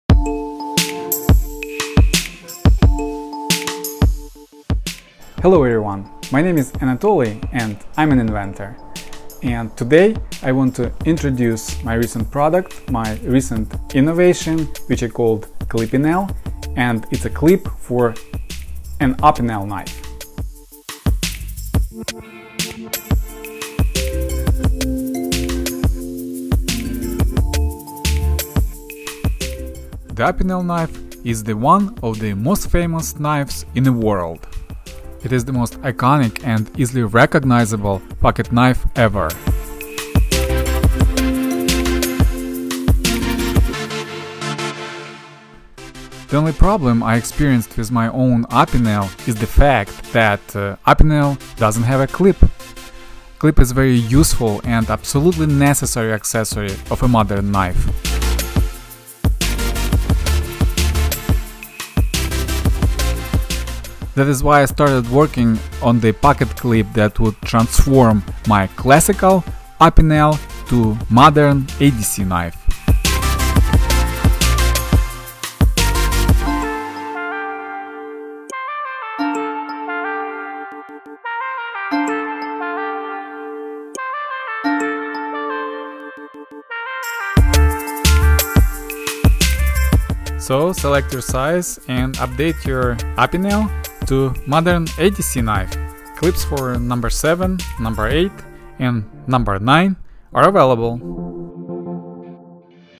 The “Clipinel” deep-carry pocket Clip for an Opinel® knife
Transform your classical Opinel ®to a modern EDC knife. Fits to #8 Opinel® Knife